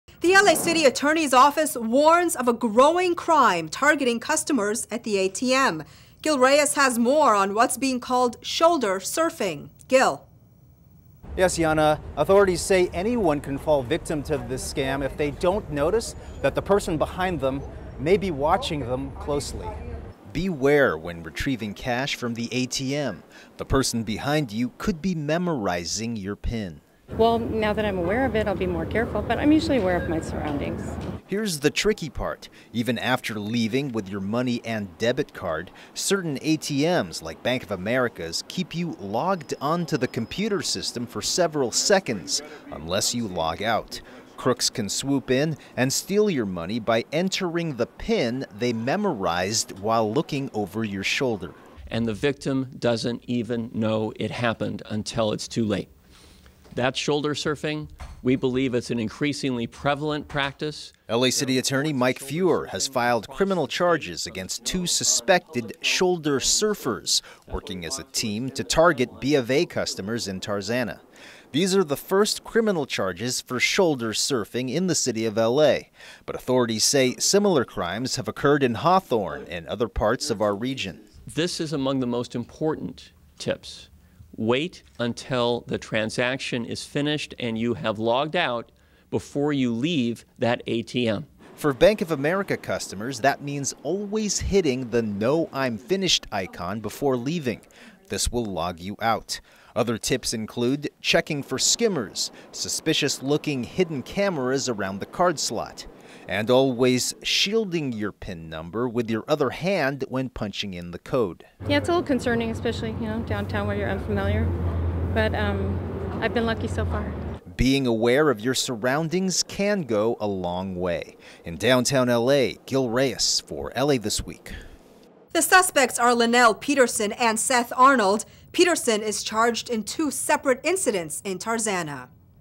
ATM Theft
Be careful and aware of your surroundings when using the ATM.  There can be someone "shoulder surfing" and taking your pin number.

Gil Reyes reports
Cameraman: Boris Gortinski